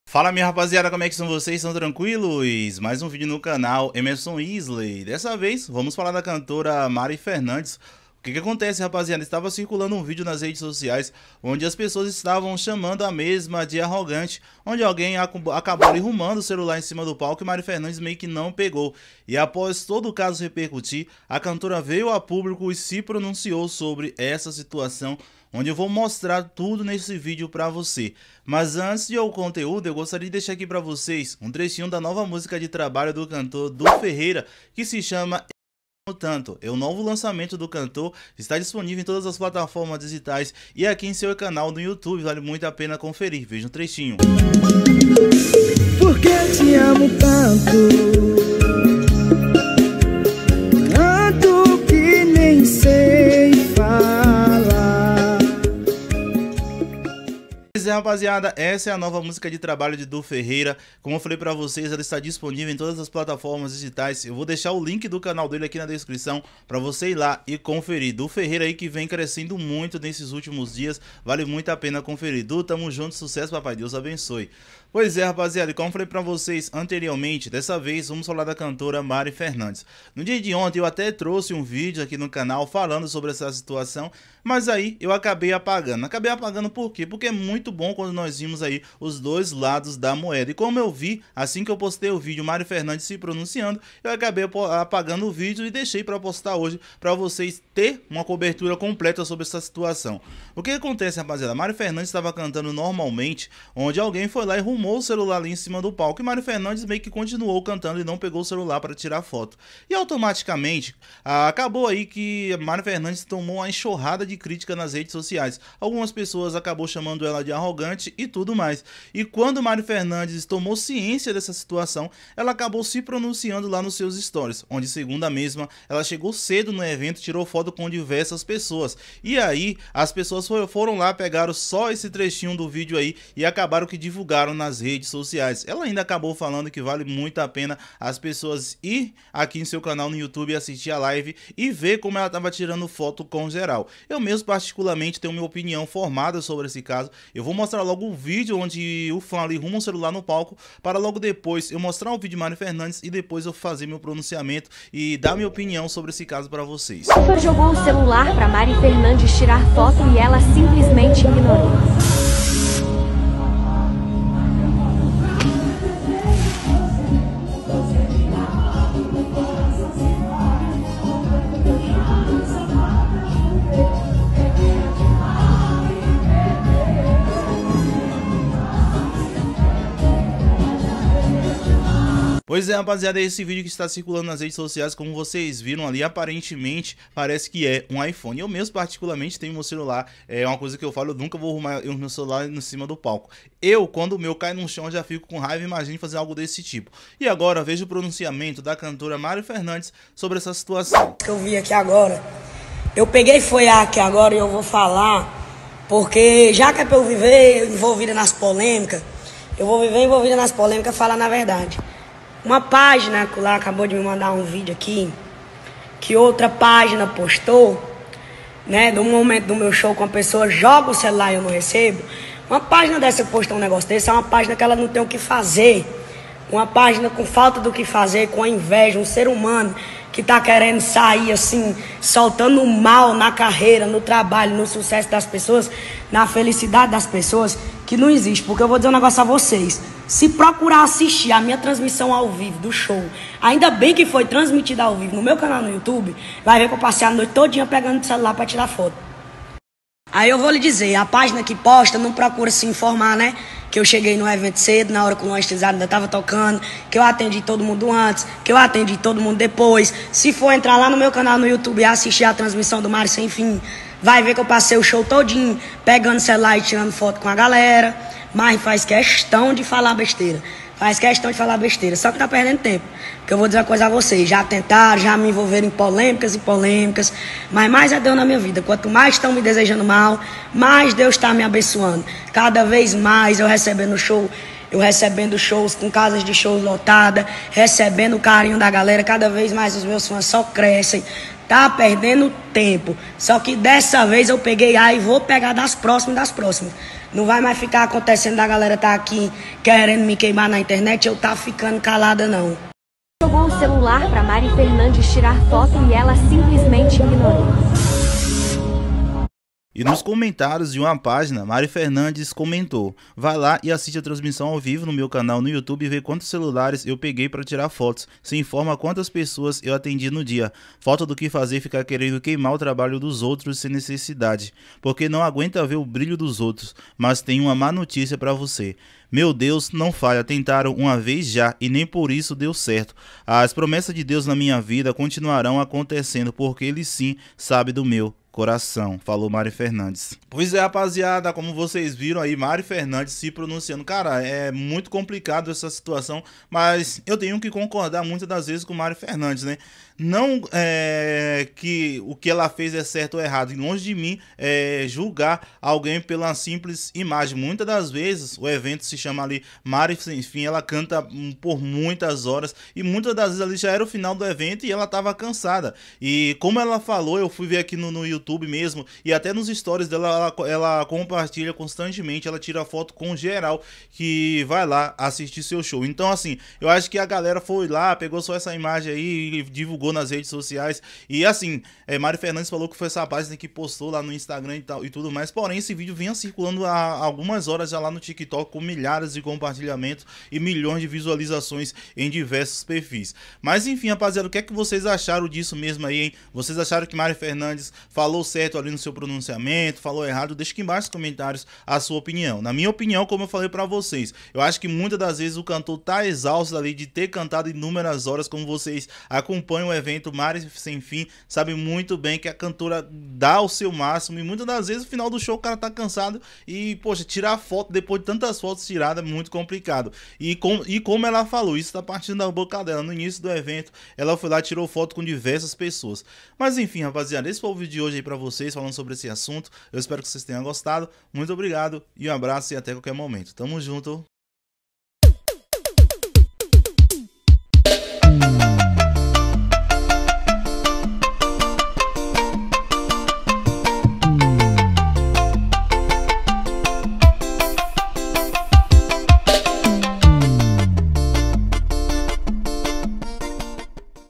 MARI FERNANDEZ FICA CHATEADA E QUEBRA SILÊNCIO APÓS SER ACUSADA DE SER ARROGANTE!
VAMOS FALAR DE MARI FERNANDEZ!